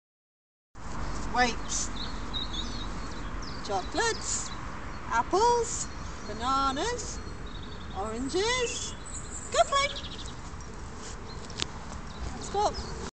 Koda Northern Inuit release command
Once the lead is off Koda must not move until she is given the release command. In this film I was testing her verbal cue, there was a dog and people in the distance..